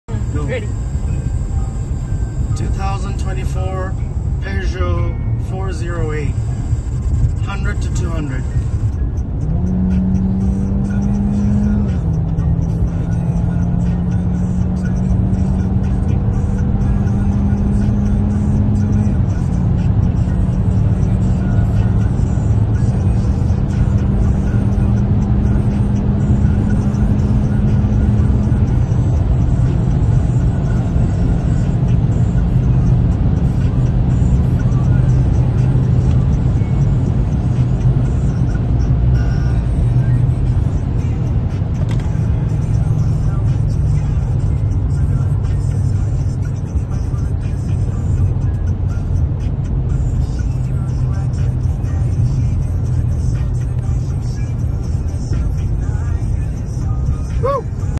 2024 Peugeot 408 100-200 km/h flat out acceleration!
Flat out acceleration with this 1.6L 4 cylinder turbo engine with 8AT, 218hp and 300Nm. Not bad for a daily driver😊 peugeot #408